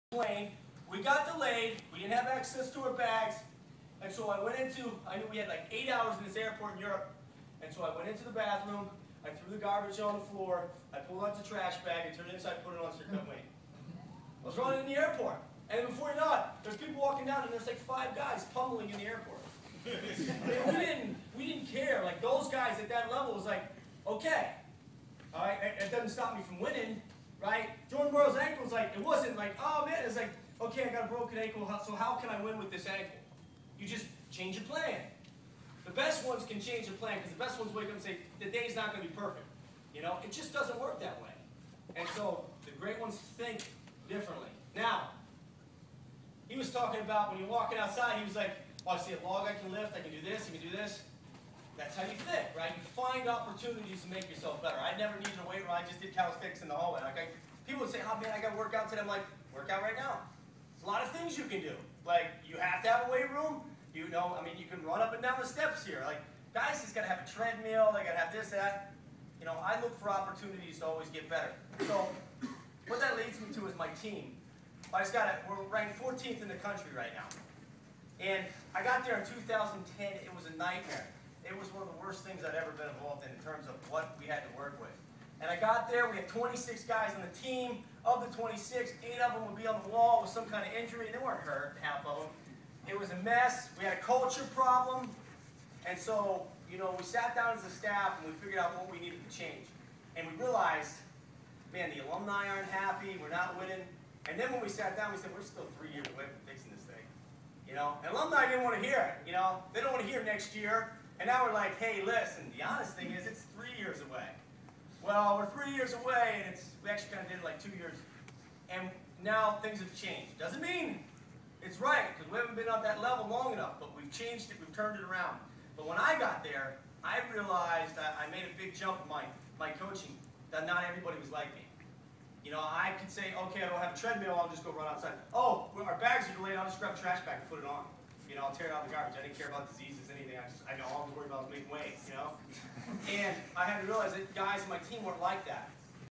The best Wrestlers/Athletes Can Adapt
Cary Kolat at the Maryland Wrestling Convention. Champions know how to deal with Adversity.